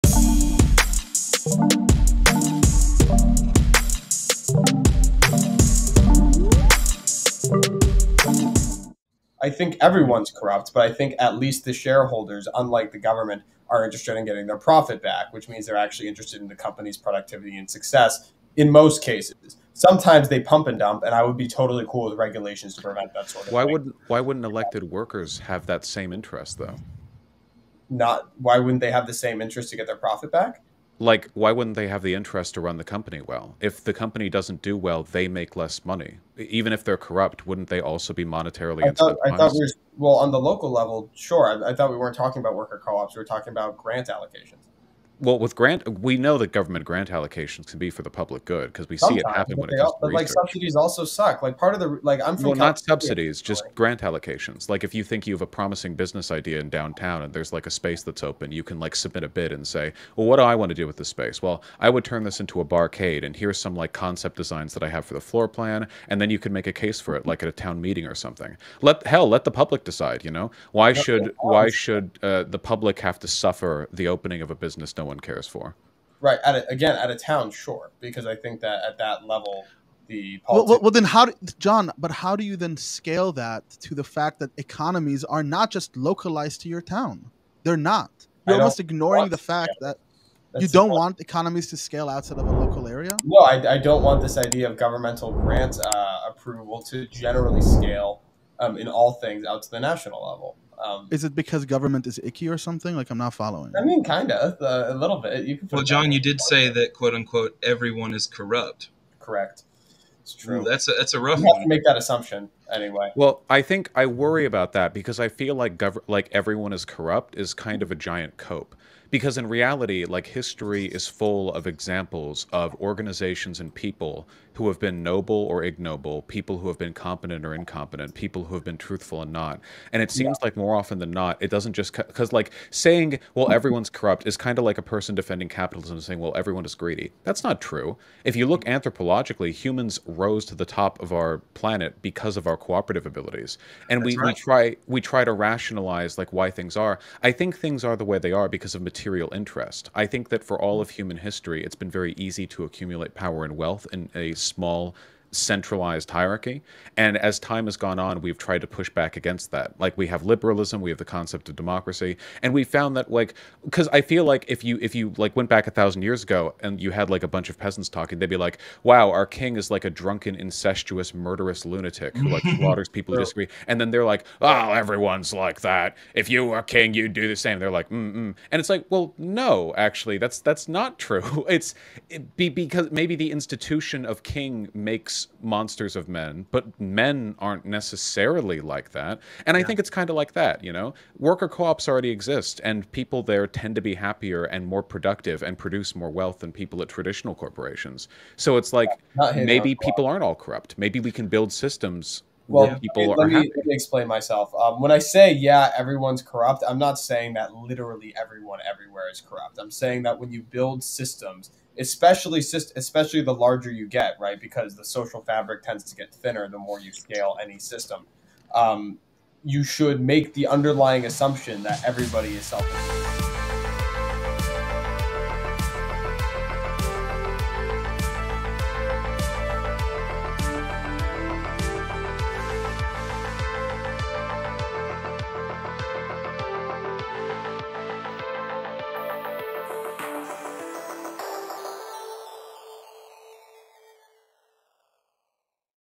SOCIALIST ECONOMICS - VAUSH vs JOHN
I had the chance to speak with Vaush on our latest episode of the Royal Rumble. We clashed in this section on the feasibility of Vaush's idea for a socialist economic system